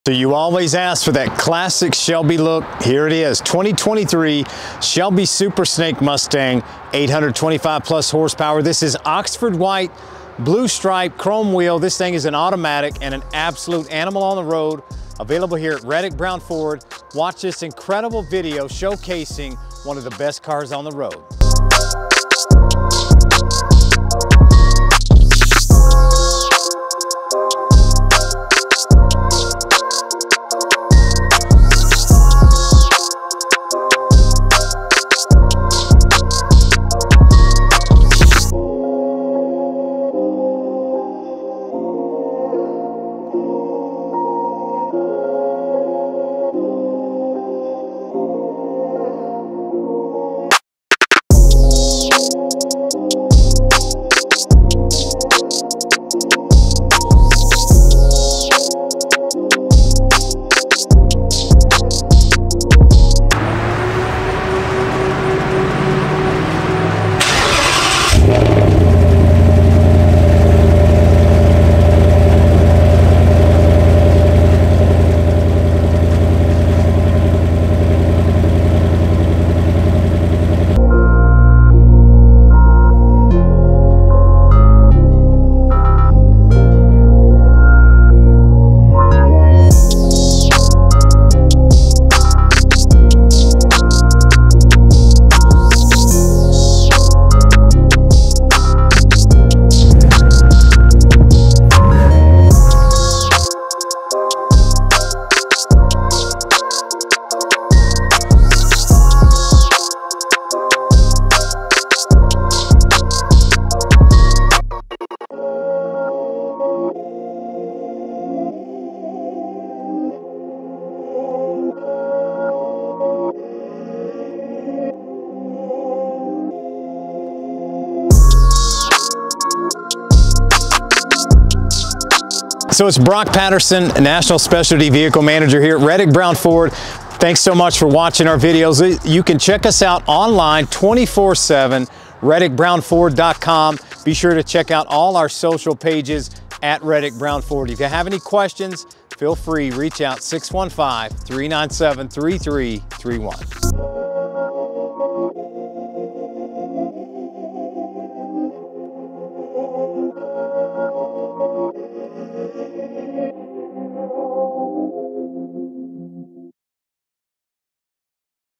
THIS Is Why You Need A 2023 Shelby Super Snake Mustang (825hp)
2023 Shelby Super Snake Mustang in Oxford White with Blue Stripes and Chrome Wheels.

PERFORMANCE
• 825 horsepower supercharger (Whipple, polished finish)
• Shelby extreme cooling (radiator, aluminum tank, heat exchanger)
• Shelby by Borla exhaust system
• Upgraded performance half shafts rated up to 1,500 HP
• Transmission cooler (automatic only)
• Short throw shifter (manual only)
• One-piece drive shaft (manual only)
• Extended hardened wheel studs, open ended lug nuts
• Ford Performance suspension upgrade
• 20” one-piece forged aluminum wheels (black or polished)
• Performance tires (summer only)
• Shelby by Brembo brake system (6 piston front, 4 piston rear)
3 year / 36,000 mile Limited Warranty
*HP ratings based on use of 93-octane fuel*

INTERIOR
• Serialized Carroll Shelby Manufacturing (CSM) badging (engine + dash)
• Shelby spec interior upgrade
• Floor mats and door sill plates
• Shelby carbon fiber gauge pod with gauges
• Shelby billet aluminium engine fluid cap set

EXTERIOR
• Shelby engineered body components:
· Aluminum Wide Body front fenders
· Metallic Super Snake ram-air hood with functional vents
· Front fascia with air managed grilles, ducts and splitter
· Side rockers with rocker wings
· Rear spoiler, tail panel and diffuser
• Super Snake striping and badging
• Tinted windows (Per State Regulations)
• Shelby louvered quarter windows

Braxton Patterson (@brrxtn)

ReddickBrownFord.com